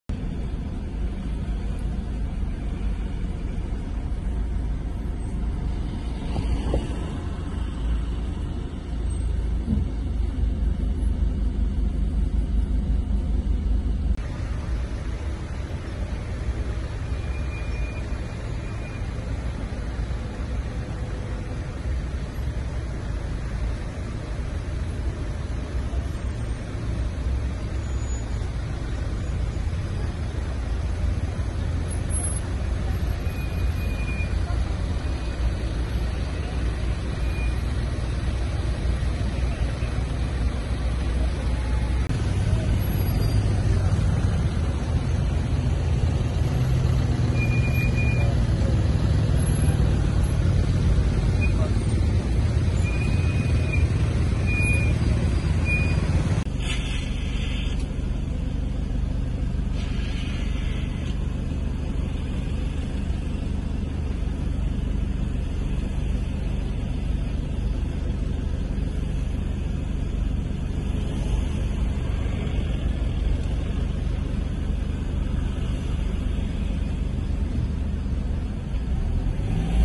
E diel dhe temperatura të larta,qytetarët drejt Vlorës për relaks,shkaktohen radhë të gjata makinash
Teksa temperaturat janë të larta dhe sot është e diel, qytetarët kanë gjetur si “shpëtim” plazhin për t'u relaksuar.

Vlora është një nga destinacionet e preferuara në bregdetin e jugut për pushuesit shqiptar dhe të huaj dhe si pasojë kjo ka shkaktuar radhë të gjata dhe fluks automjetesh në rrugët e qytetit.